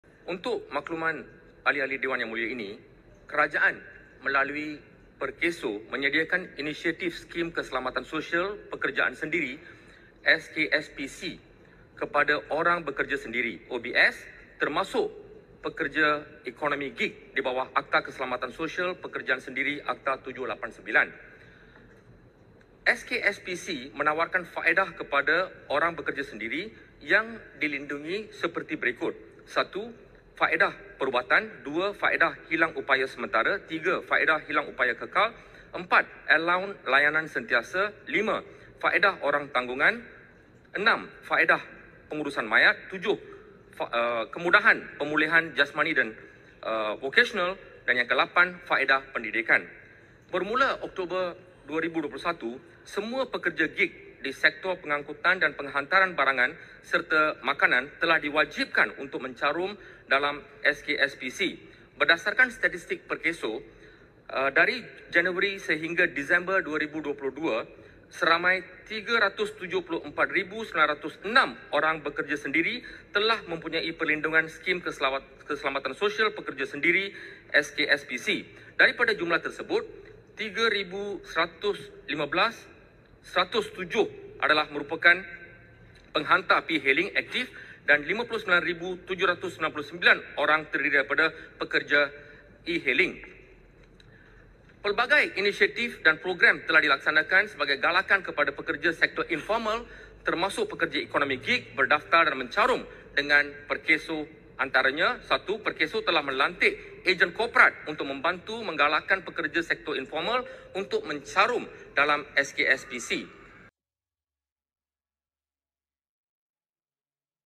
Dasar wajibkan pekerja sendiri carum PERKESO dilaksana secepat mungkin
Kementerian Sumber Manusia (KSM) akan memastikan dasar kerajaan yang mewajibkan semua individu bekerja sendiri termasuk pekerja gig, mencarum dengan Skim Keselamatan Sosial Pekerjaan Sendiri (SKSPS), dapat dilaksanakan dalam waktu terdekat.

Menteri Sumber Manusia V Sivakumar berkata dalam mewajibkan jaminan perlindungan insurans bagi semua sektor termasuk sektor ekonomi gig, Pertubuhan Keselamatan Sosial (PERKESO) menghadapi pelbagai cabaran antaranya masih tiada dasar atau satu perundangan khusus untuk menjaga kebajikan pekerja ekonomi gig.